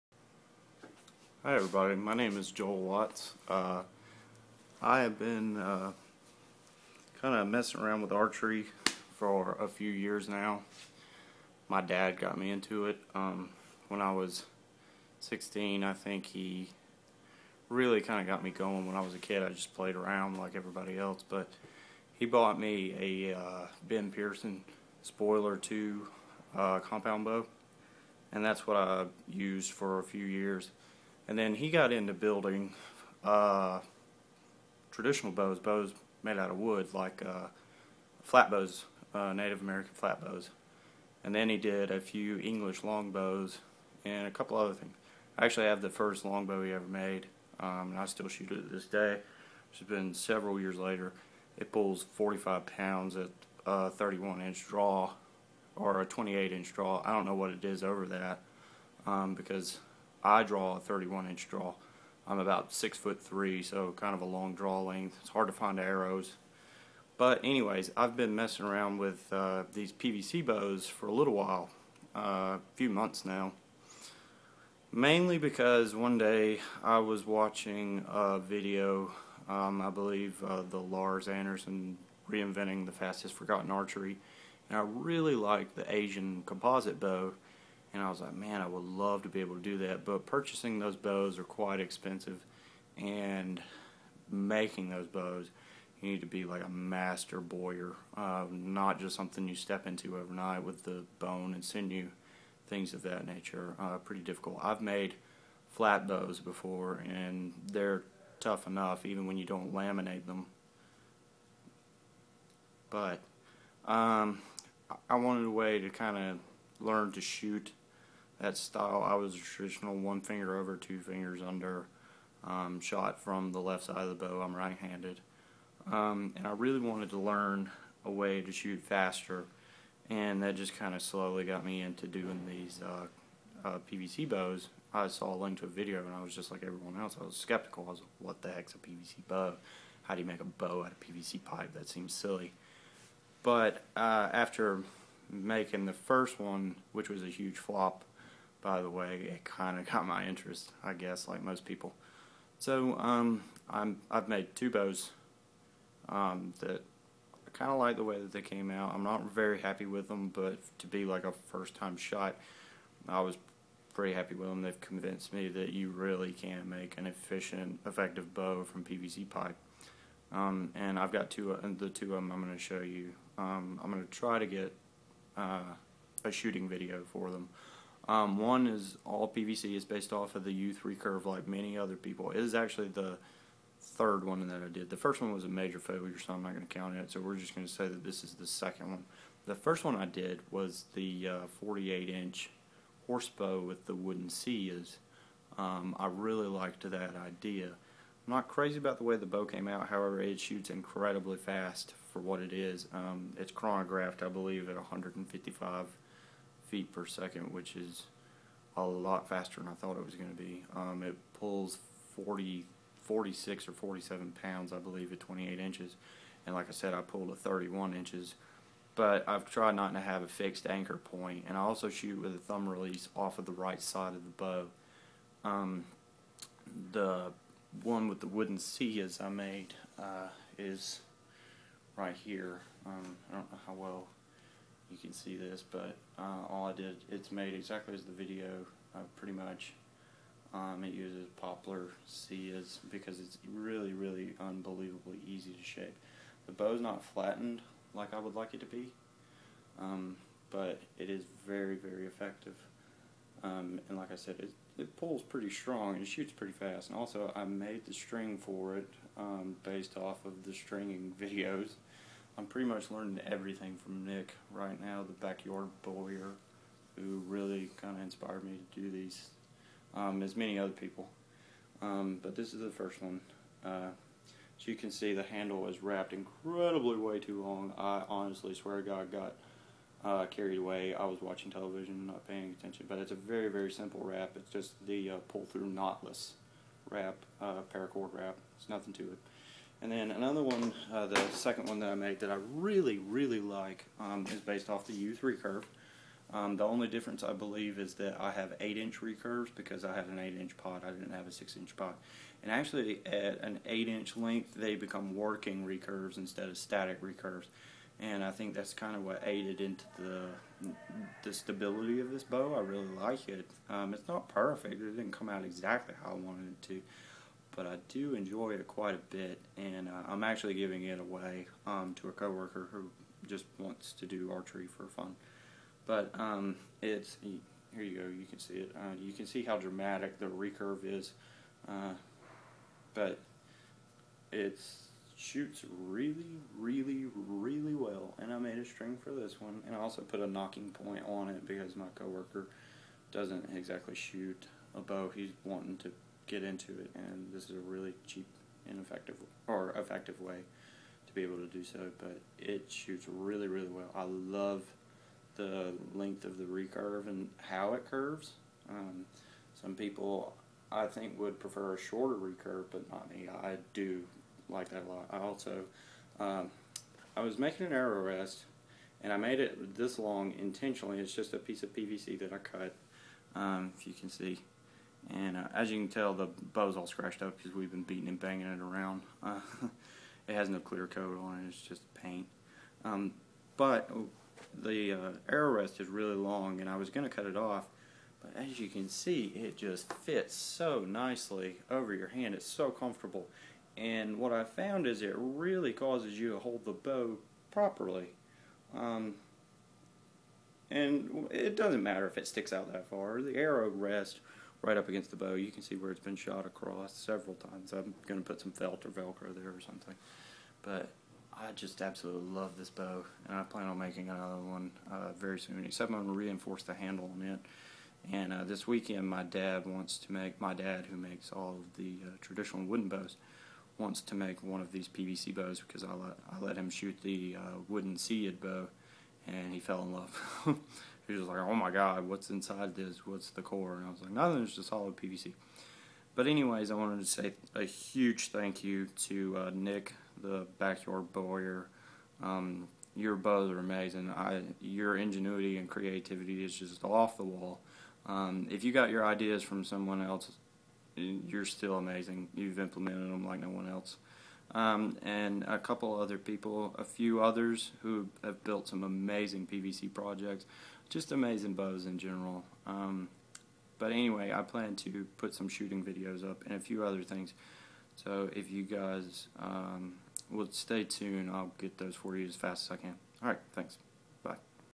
A huge thank you to the backyard bowyer
My first two PVC bows. A wood siyah'd bow: 46#'s @ 31" 150 fps. And a youth recurve 33#'s @ 28" 130 fps.